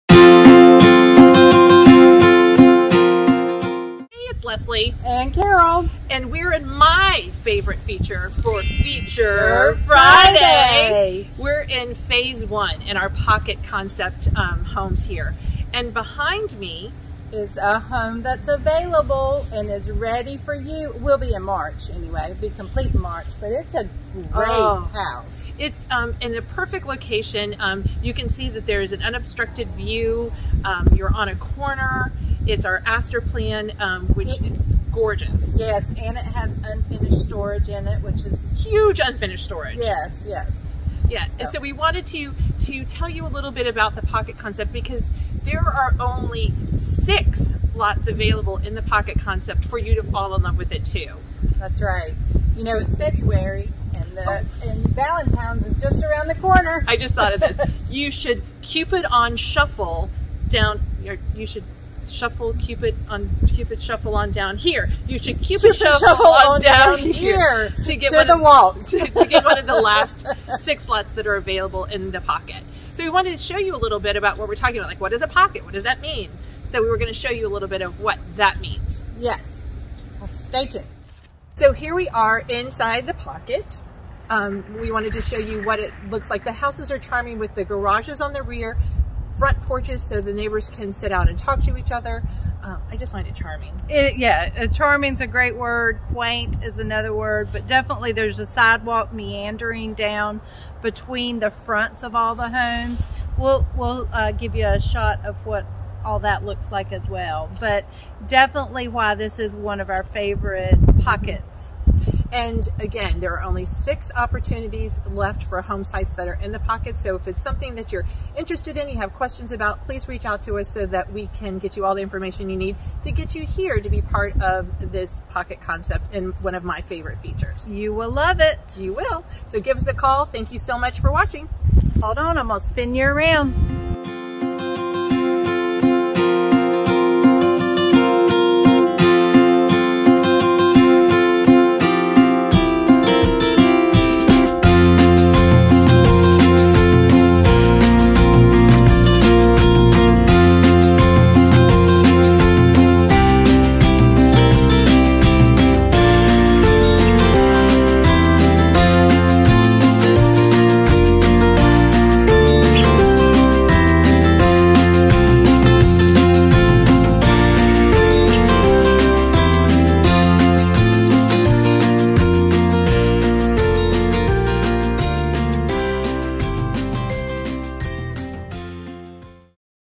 Feature Friday - Pocket Homes
Welcome To The Walk at East Village! A premier community for active adults, 55 and up.

In this video, we’re taking you outside, to the “Pocket Homes” inside The Walk at East Village, and teach you what a Pocket Home is.

What are Pocket Homes?
Pocket homes are small clusters of homes gathered around a shared open space. They take the form of a garden courtyard with landscaped lawns and a walking path surrounded by homes. These clusters have 8-12 homes within the larger neighborhood.

People Out Front, Cars in Back
You can walk out your door and take a short walk around your neighborhood, greeting your front porch community along the way. Your cars stay tucked behind your home in a garage with landscaped alley access.